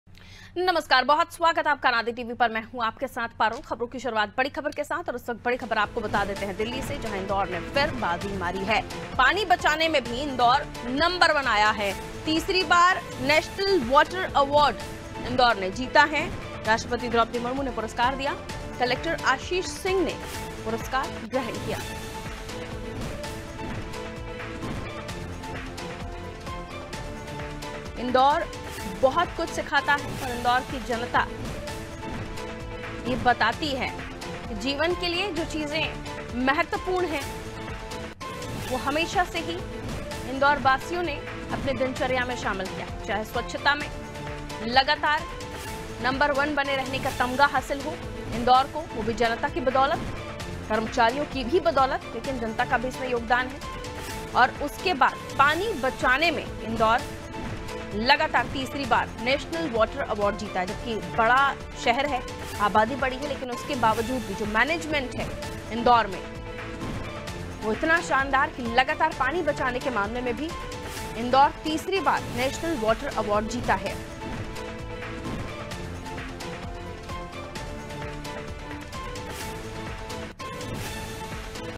Delhi : इंदौर ने फिर मारी बाज़ी, तीसरी बार जीता नेशनल वाटर अवार्ड | Breaking News || Anaadi Tv
Delhi : इंदौर ने फिर मारी बाज़ी, तीसरी बार जीता नेशनल वाटर अवार्ड | Breaking News ||

HATHWAY- 231
SITI- 356
SKY- 441
ACN- 231
AIRTEL - 379
JIO TV, MX PLAYER, VI